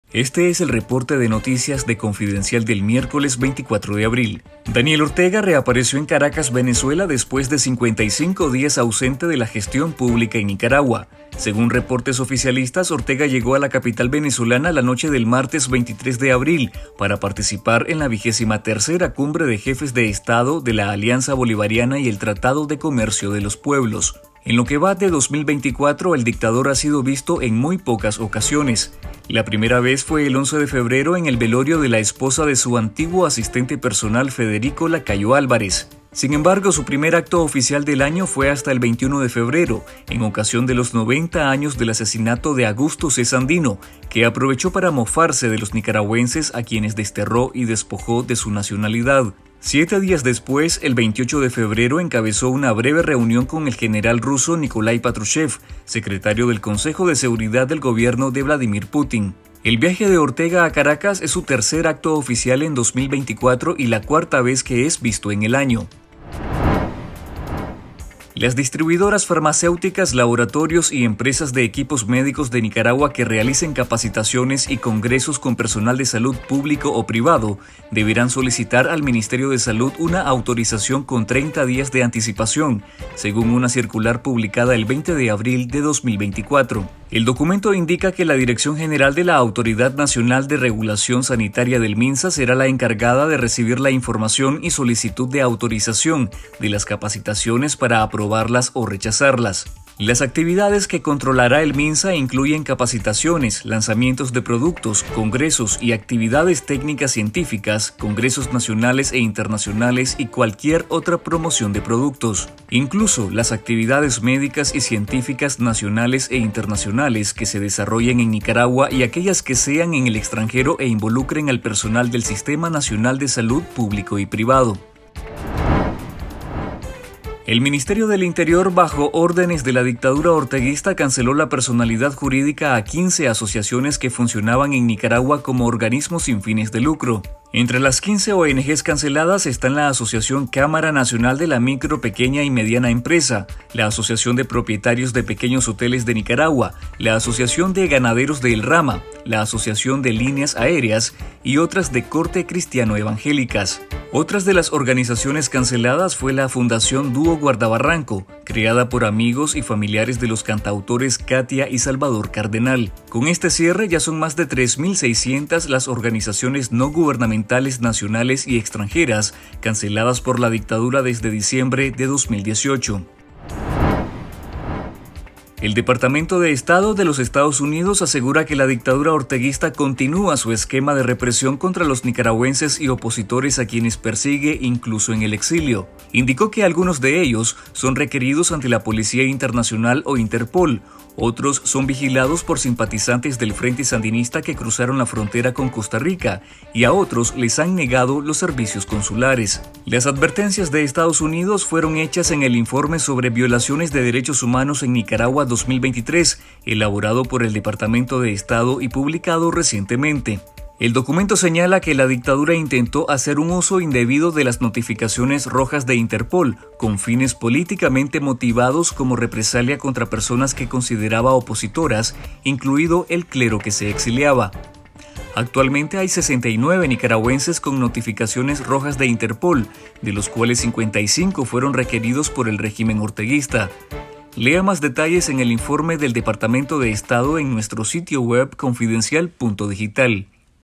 Daniel Ortega reaparece en Venezuela, para cumbre del ALBA
Daniel Ortega reapareció en Caracas, Venezuela, después de 55 días ausente de la gestión pública en Nicaragua. Según reportes oficialistas, Ortega llegó a la capital venezolana, la noche del martes 23 de abril, para participar en la vigésima tercera cumbre de jefes de Estado de la Alianza Bolivariana y el Tratado de Comercio de los Pueblos.

En otras noticias:
- Minsa controlará capacitaciones del personal de Salud público y privado.
- Ordenan cierre de Asociación de Pequeños Hoteles y otras 14 oenegés.
- Estados Unidos advierte “uso indebido” de las alertas de la Interpol.